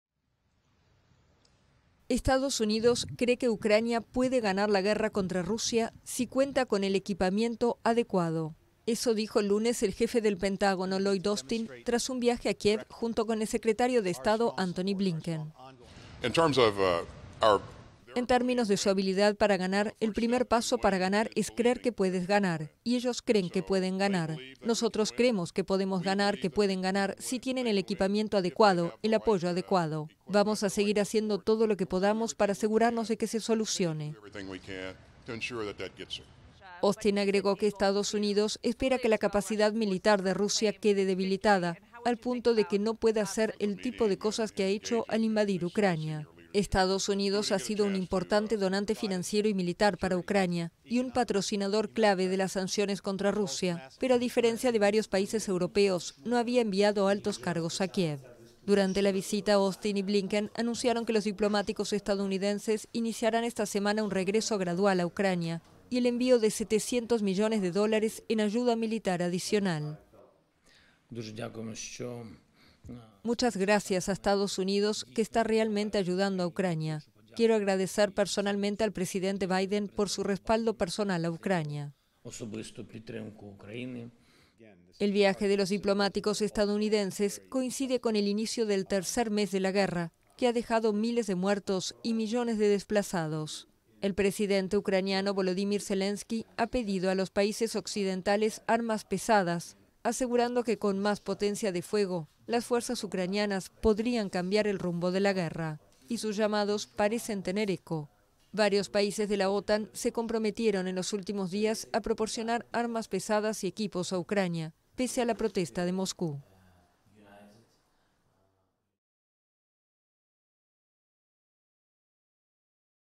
Armamento ucraniano: EEUU cree que Ucrania puede derrotar a Rusia con "equipamiento adecuado"
Estados Unidos cree que Ucrania puede ganar la guerra contra Rusia si cuenta con el "equipamiento adecuado", dijo el jefe del Pentágono, Lloyd Austin, tras un viaje a Kiev.